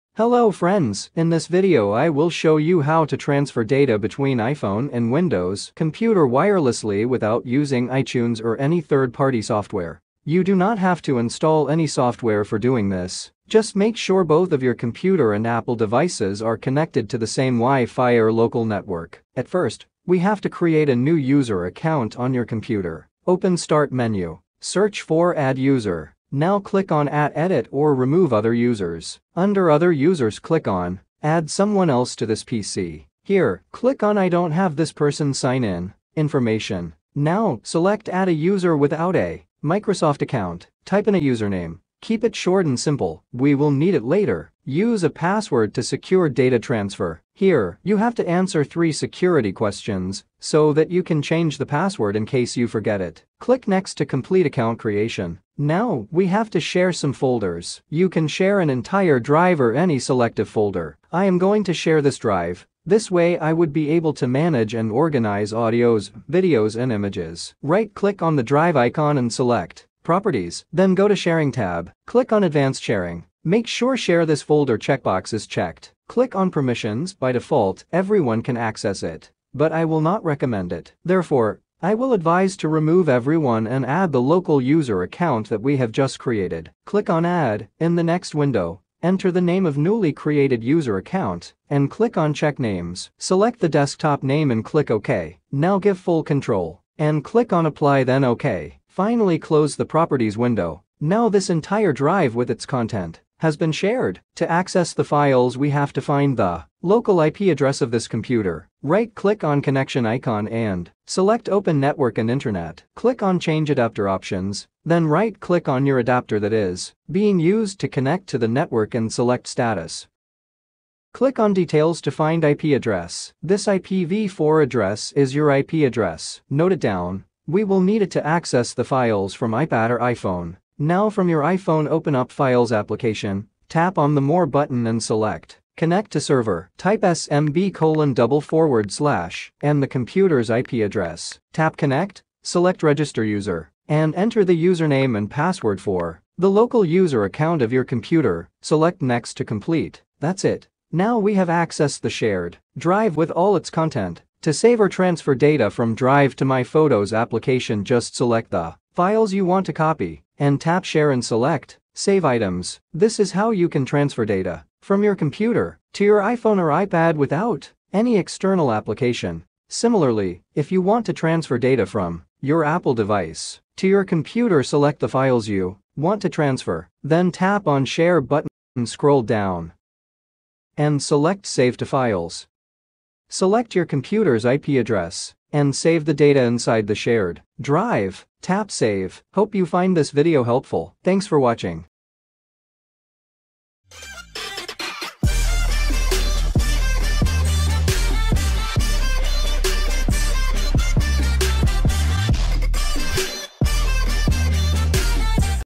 Wireless File Transfer Between Windows PC and iPhone/iPad | No Software Required | Bypass iTunes
Hello friends, in this video I will show you how to transfer data between iPhone and windows computer wirelessly without using iTunes or any third party software. You do not have to install any software for doing this.

Just make sure both of your Computer and apple devices are connected to the same Wi-Fi or local network.

After sharing any folder or drive from your computer, to save or transfer data from computer to my photos application of your iPhone just select the files you want to copy and tap share then select save items.